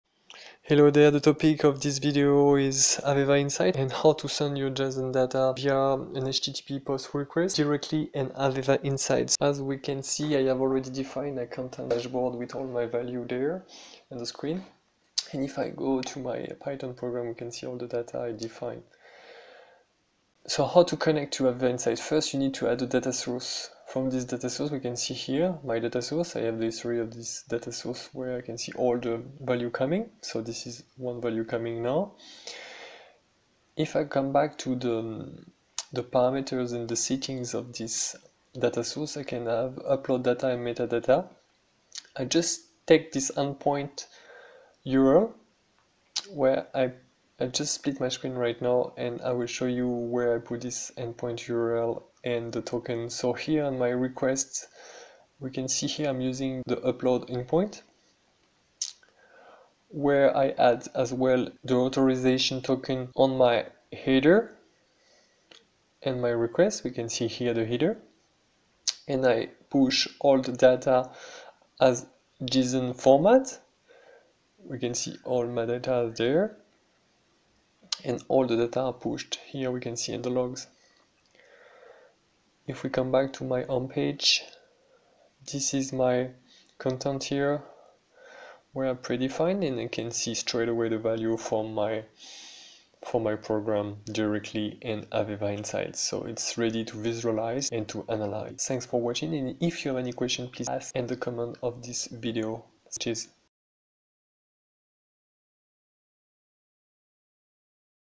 Send Data to AVEVA Insight | JSON
How to upload and send data to AVEVA Insight using Python, HTTP POST request and JSON data sources. Connect and share your IoT data to AVEVA Insight. IIoT.

AVEVA